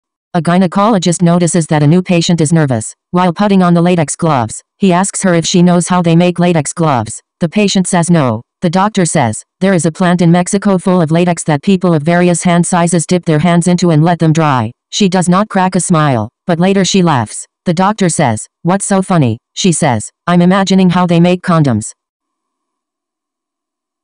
Jokes - A gynecologist notices that a new patient is nervous. While putting on the latex gloves, he
Joke :
A gynecologist notices that a new patient is nervous. While putting on the latex gloves, he asks her if she knows how they make latex gloves. The patient says no. The doctor says, "There is a plant in Mexico full of latex that people of various hand sizes dip their hands into and let them dry. She does not crack a smile, but later she laughs. The doctor says, "What's so funny?" She says, "I'm imagining how they make condoms."